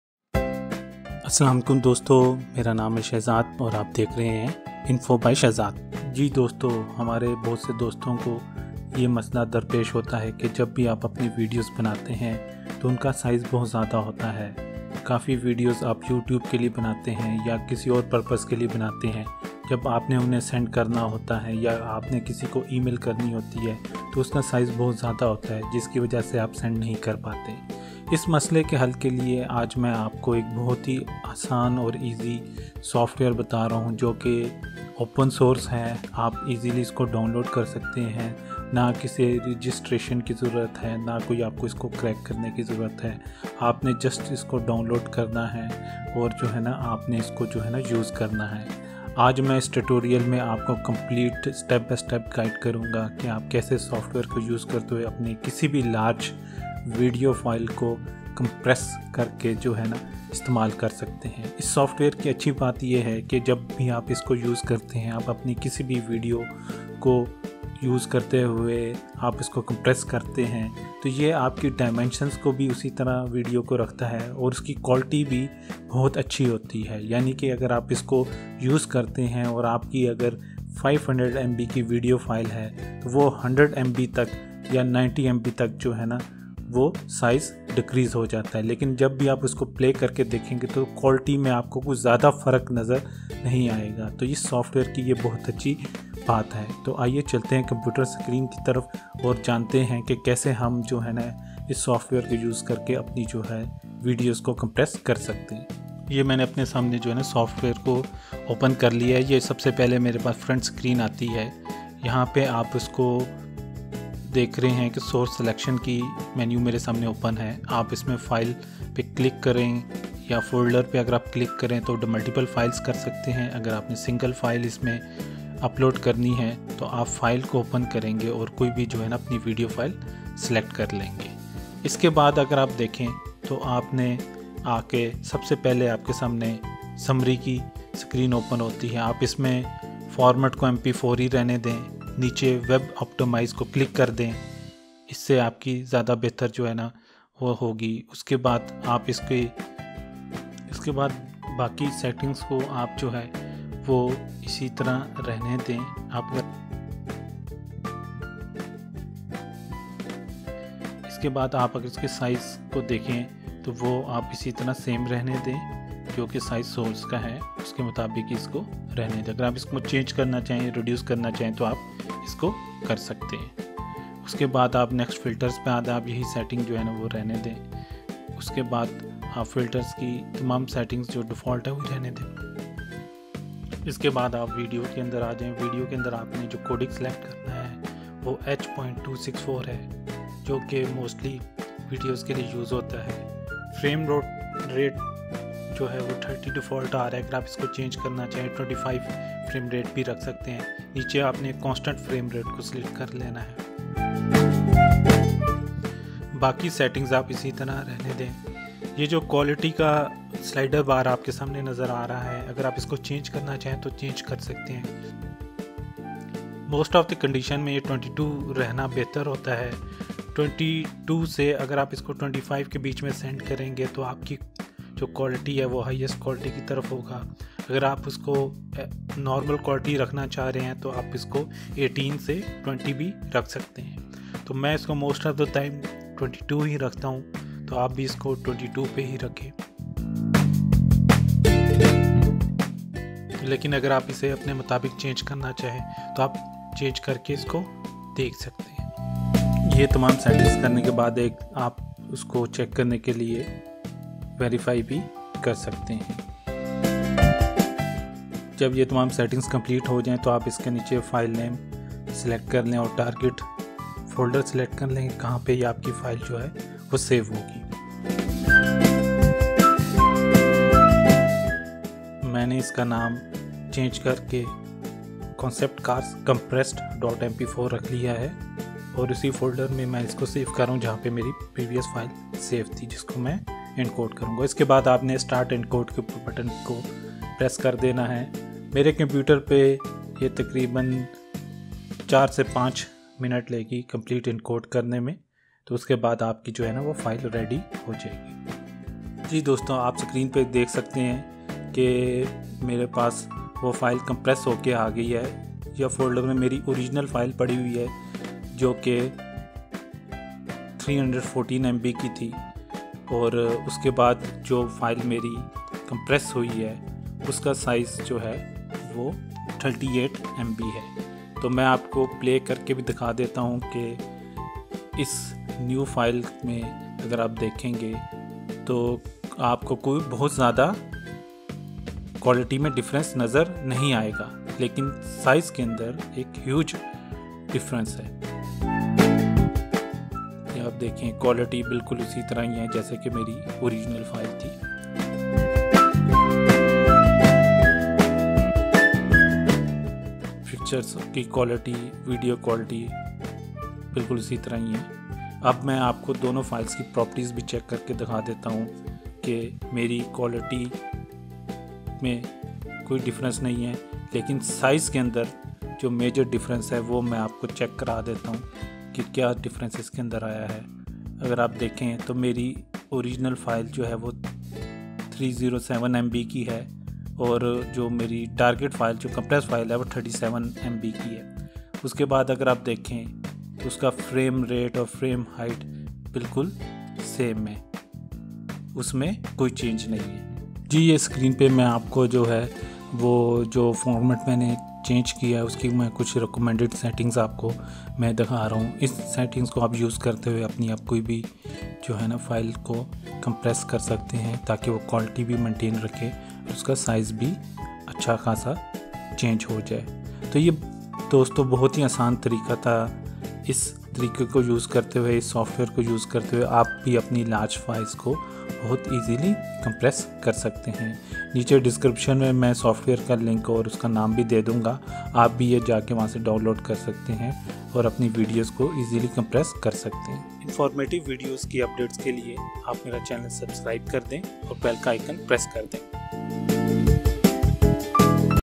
Compress Large Videos Without Losing Quality – Urdu
This video is about How to compress large video file without losing quality. I will explain in urdu how we can use this software name handbrake to compress and reduce the size of video file easily. I will compare both files and explain you the difference after compress completed.

Software name: Handbrake

Topics Covered

Compress large video file without losing quality

How we can use this software to compress and reduce the size of video file

Compare both files and explain you the difference after compressed files